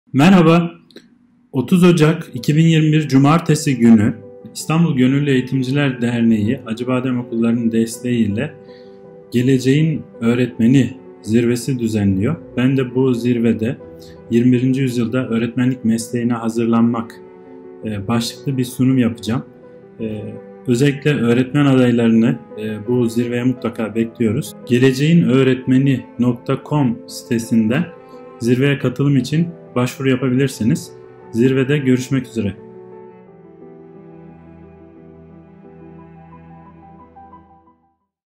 Doç. Dr. Yusuf ALPAYDIN Geleceğin Öğretmeni Zirvesinde
İstanbul Gönüllü Eğitimciler Derneği (İGEDER) ve Özel Acıbadem Okulları tarafından organize edilen Geleceğin Öğretmeni Zirvesine hoş geldiniz.
Geleceğin Öğretmeni Zirvesi, eğitim, fen-edebiyat ve ilahiyat fakültelerindeki öğrencilerin bir araya geldiği interaktif bir etkinliktir. Zirve sonrasında ise adayları “Geleceğin Öğretmeni Akademisi” programı bekliyor. Adayların mesleki birikimlerini arttıracağı eğitimleri barındıran akademi programına, zirveye katılanlar öncelikli aday olacaklardır.